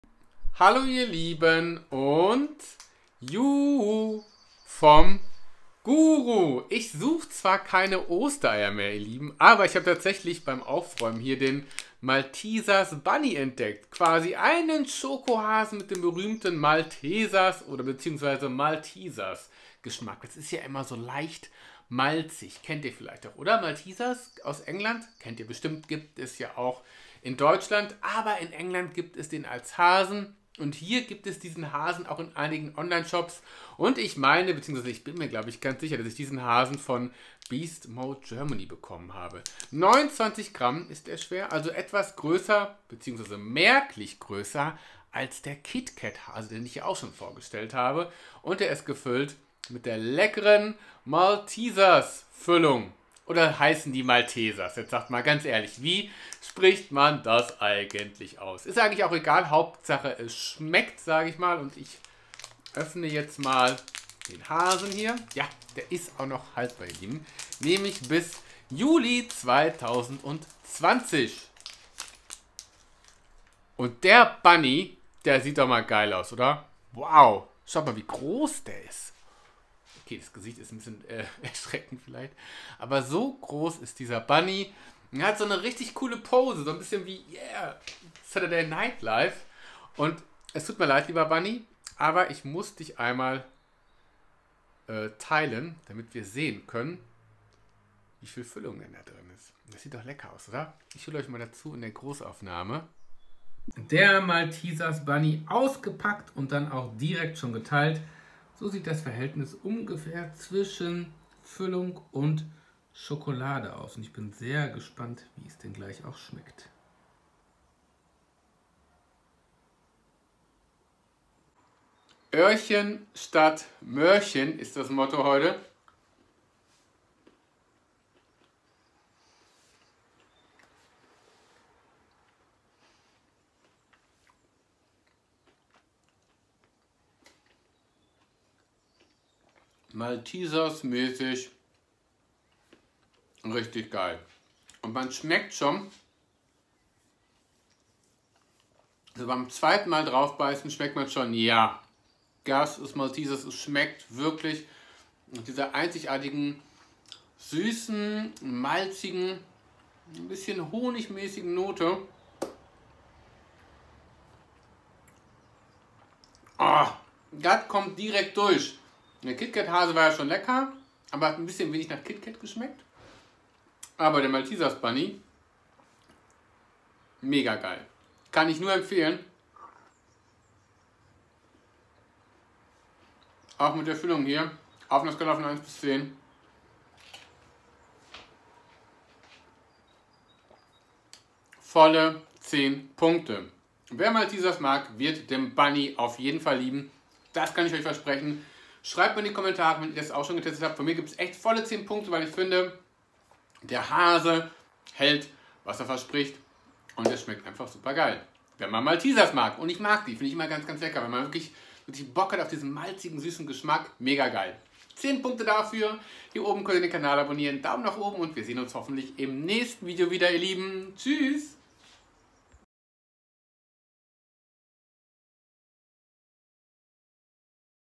Maltesers Bunny im Test: So schmeckt der 29g Osterhase aus England!
Heute teste ich für euch den Maltesers Bunny aus England für euch und verrate dabei nicht nur wie er schmeckt, sondern wo man ihn auch in Deutschland kaufen kann. Viel Spaß beim Video und frohe Ostern.